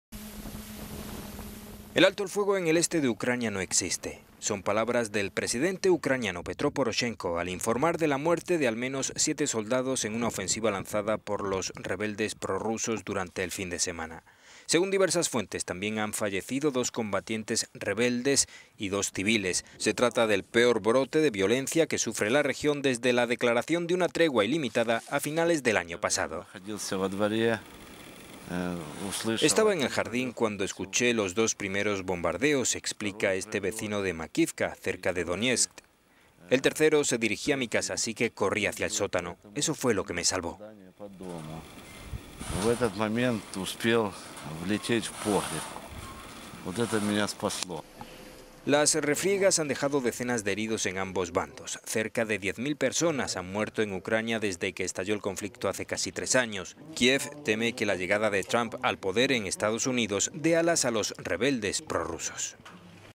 Al menos 11 muertos en un brote de violencia bélica en el este de Ucrania
El alto el fuego en el este de Ucrania "no existe". Son palabras del presidente ucraniano, Petró Poroshenko, al informar de la muerte de al menos siete soldados en una ofensiva lanzada por los rebeldes prorrusos durante el fin de semana. Según diversas fuentes, también han fallecido dos combatientes rebeldes y dos civiles. Se trata del peor brote de violencia que sufre la región desde la declaración de una tregua ilimitada a finales del año pasado.